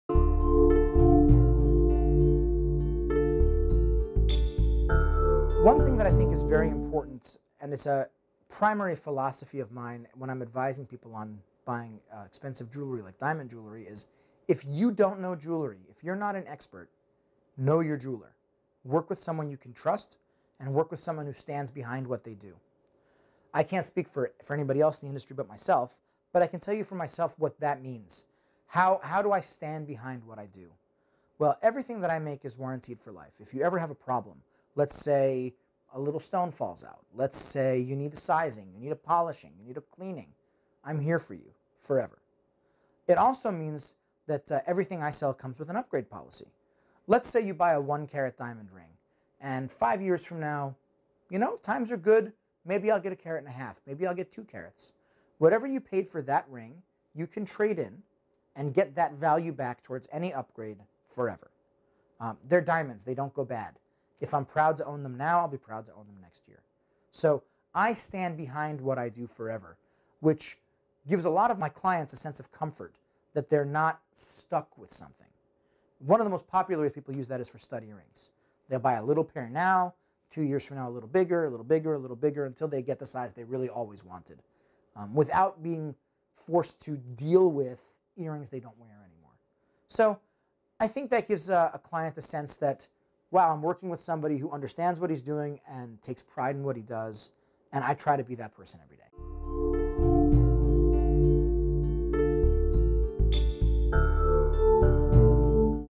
The Concierge Diamond Guarantee
Everything that we make is warranted for life. If you ever have a problem--a little stone falls out, you need sizing, you need polishing, you need cleaning, Concierge Diamonds is here for you, forever.

Everything we sell comes with an upgrade policy. Let's say, you buy a one carat diamond ring.  Whatever you paid for that ring, you can trade in and get that value back towards a bigger diamond, forever.